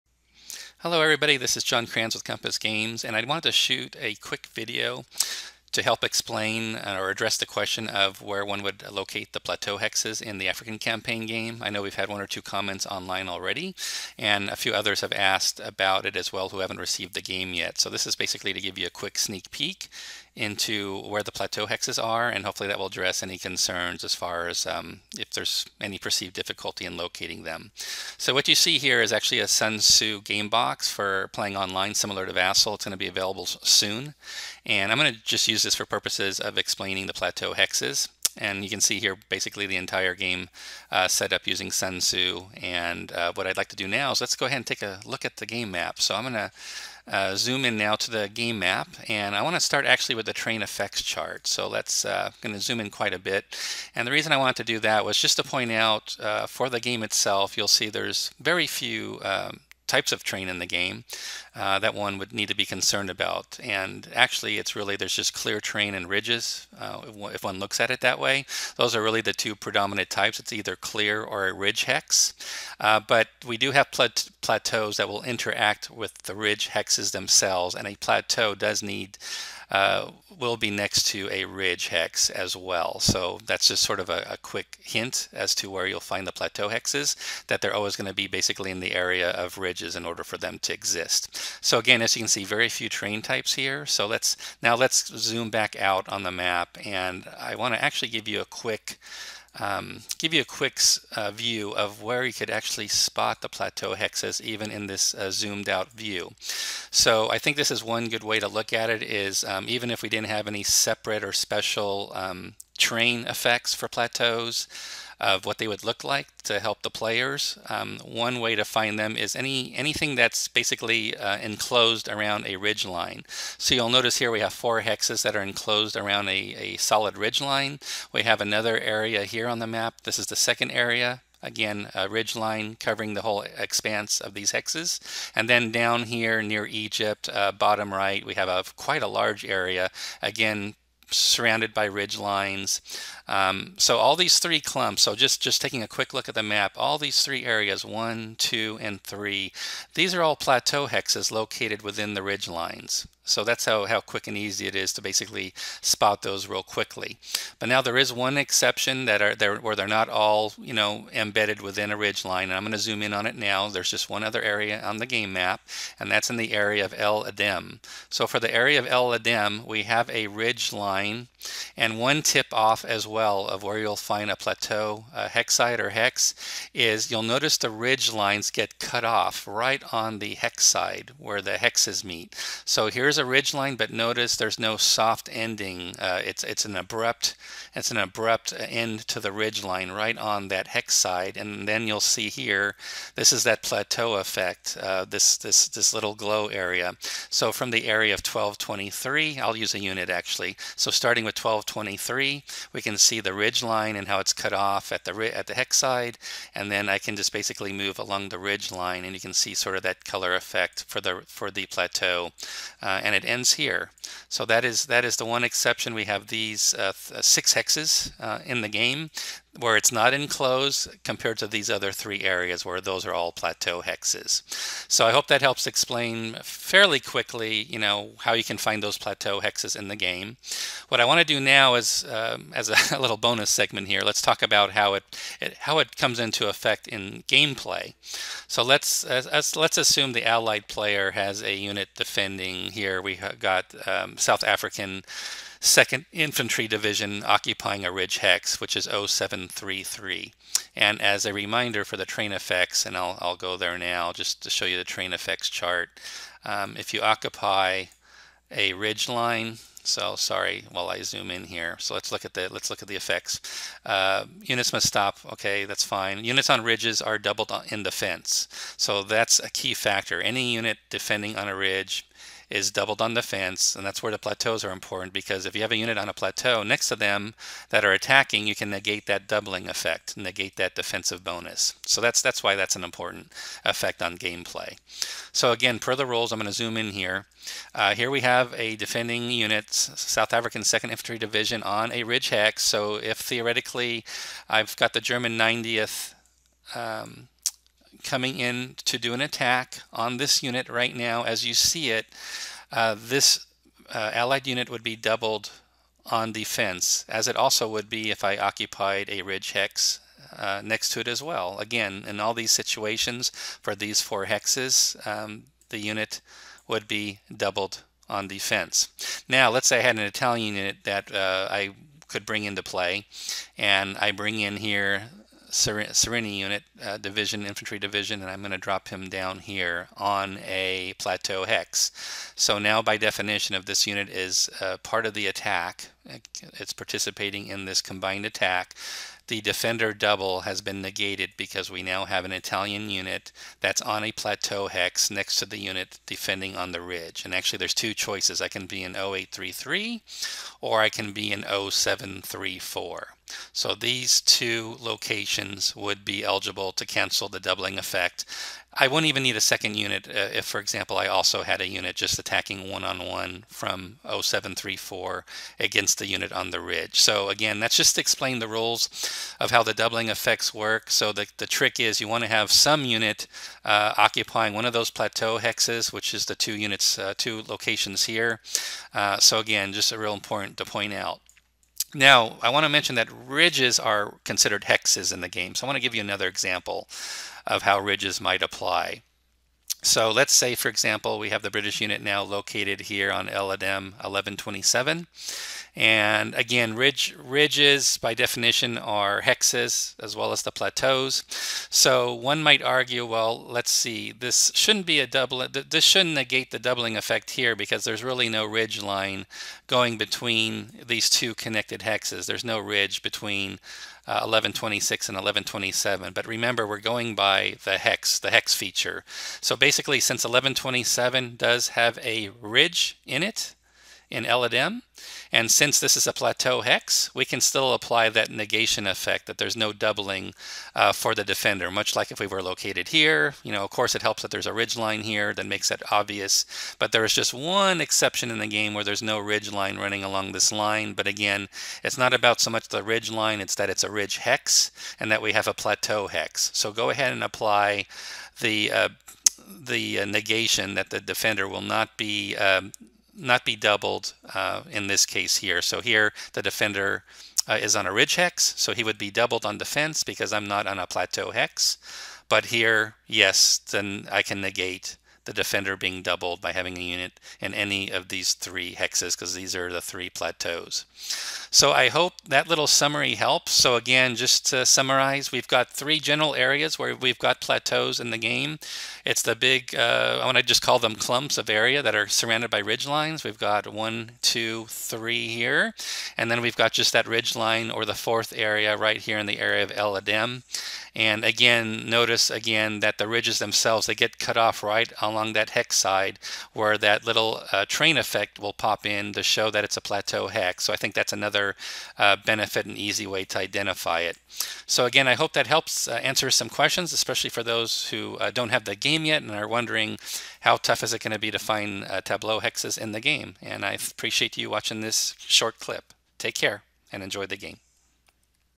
Tutorial: Plateau hexes in The African Campaign
Here is a brief overview of Plateau hexes in The African Campaign, Designer Signature Edition.